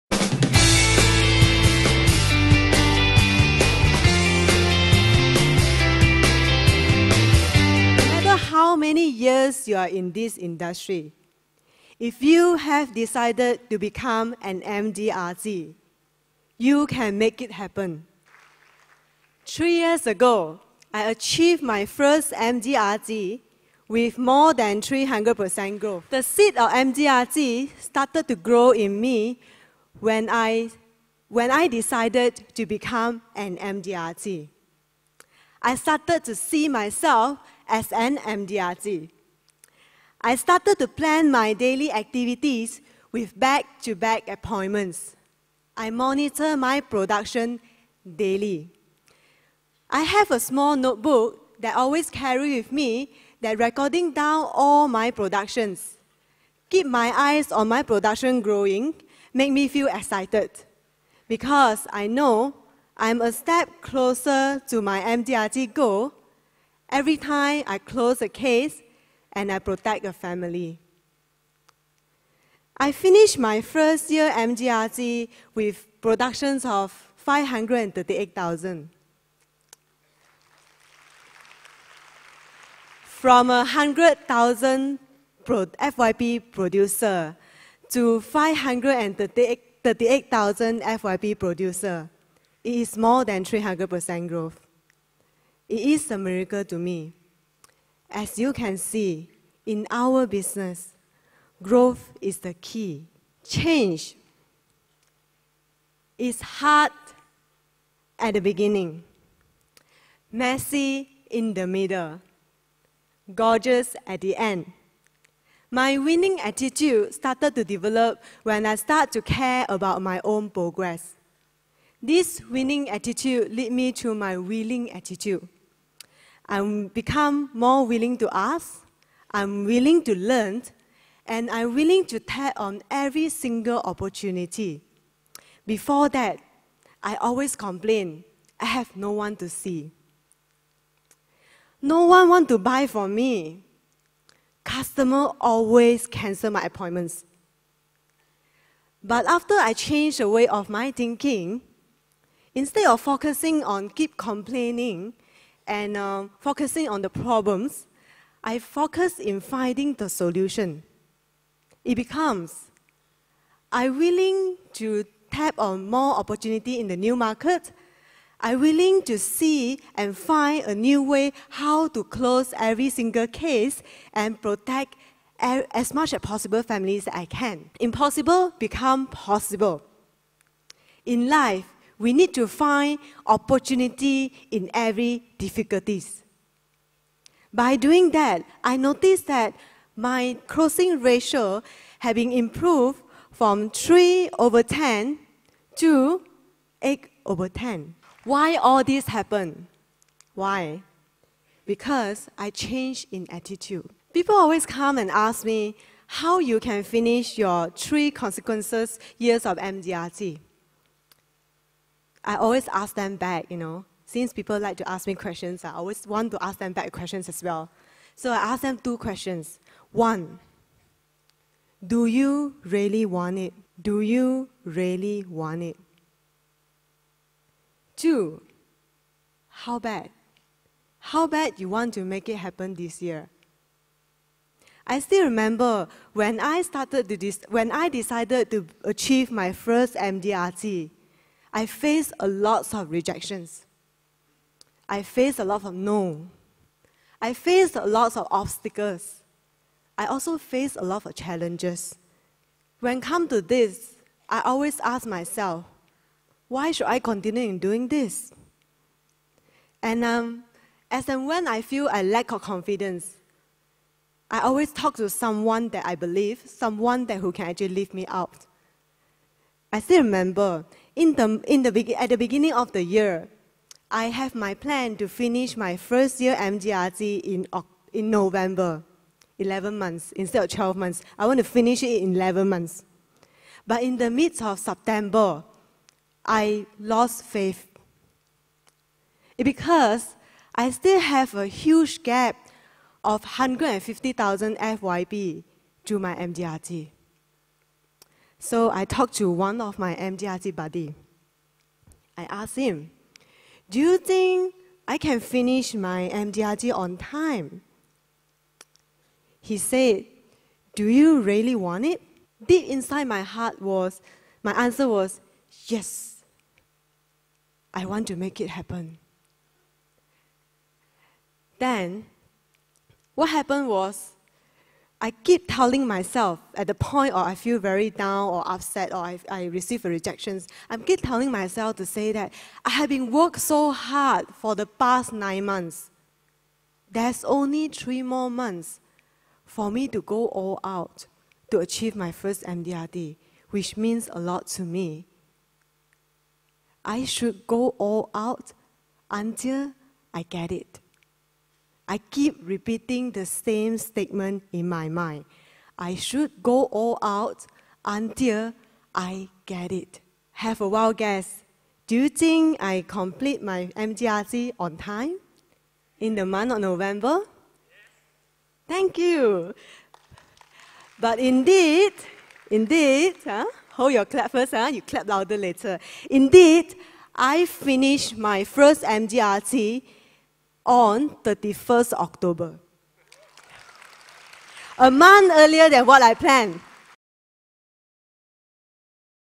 Malaysia MDRT Day 2018 - Michelle Yeoh 1/2
Her struggles with achieving MDRT and how she grows by achieving it.